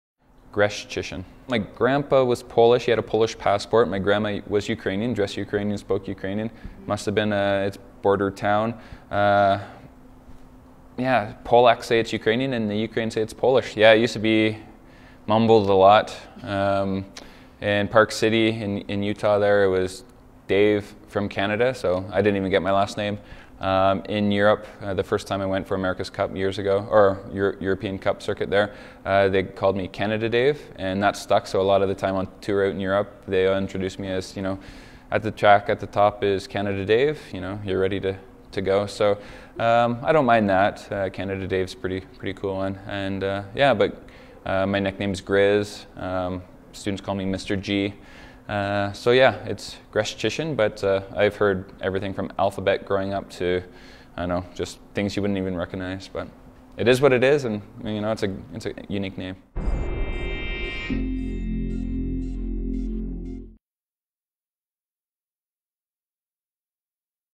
Why Olympic skeleton racer Greszczyszyn gets called ‘Canada Dave’
Dave Greszczyszyn says faced with his “unique” last name, sliding track announcers sometimes give up. The Olympic skeleton racer says some people in the U.S. just call him “Dave from Canada.” (The Canadian Press)

For more on the 2018 Winter Olympics visit the Toronto Star